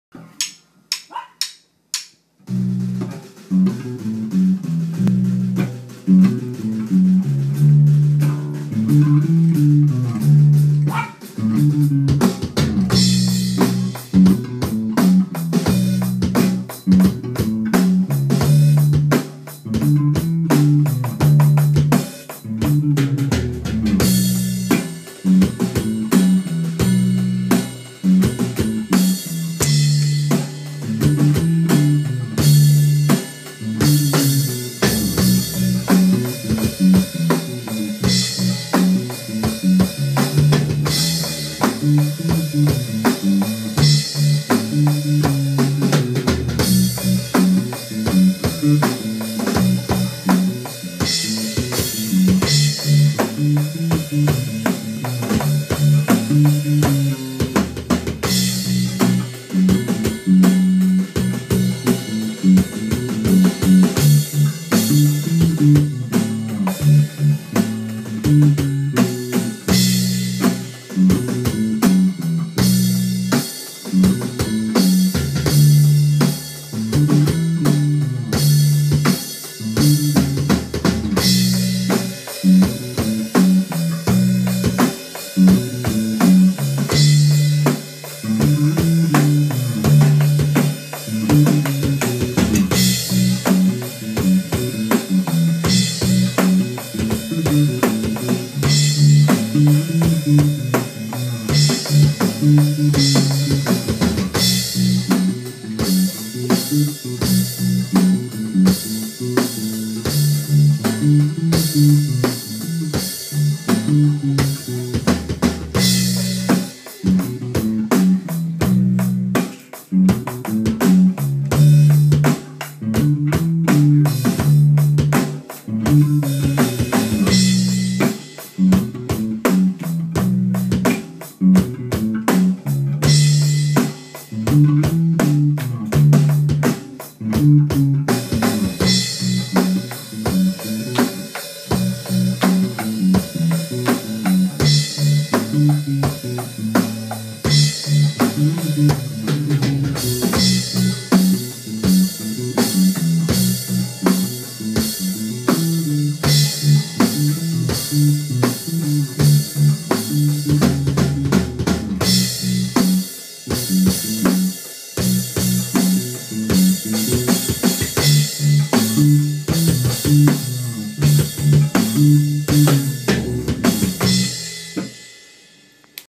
Me and Michael jamming
Me and Michael jamming together for the first time.. he came up with that awesome bass line ;)